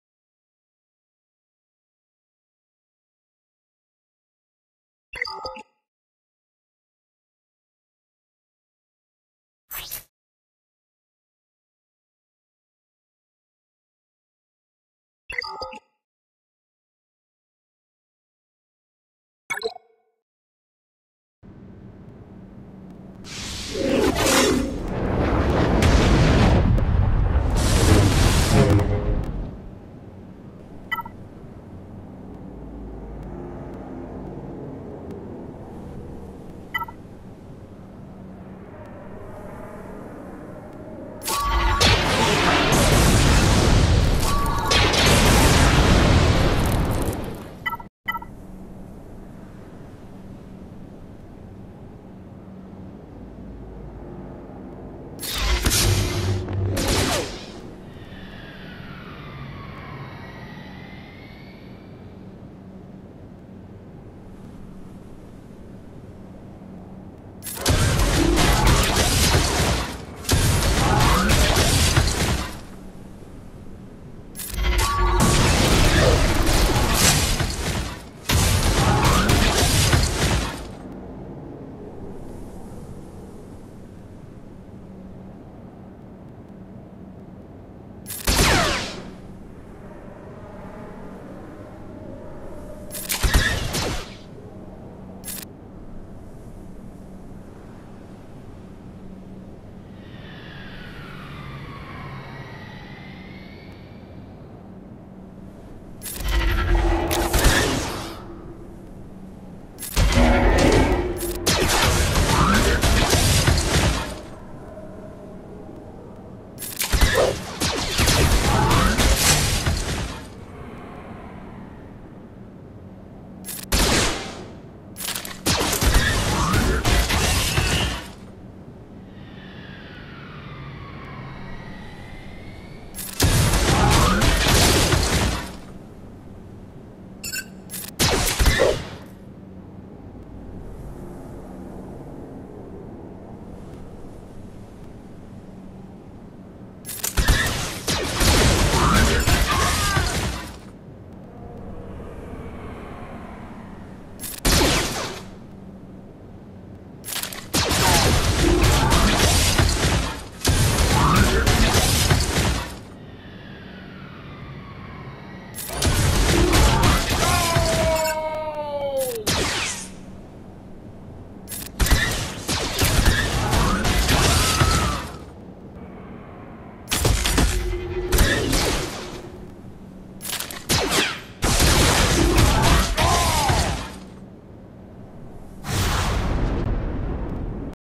Veers/Piett/Dark/Range/Iden (prot up, O buffs) vs. LV/RG/Maul/DV/Piett (UFU D, 50% D): 65 banners
He had a Piett over 350 speed, so I knew he would go before Iden. But so long as my Piett could Daze Maul (and Piett), I could prevent Maul's 2nd Frenzy from being triggered (and prevent Piett's passive TM gains). This worked out perfectly.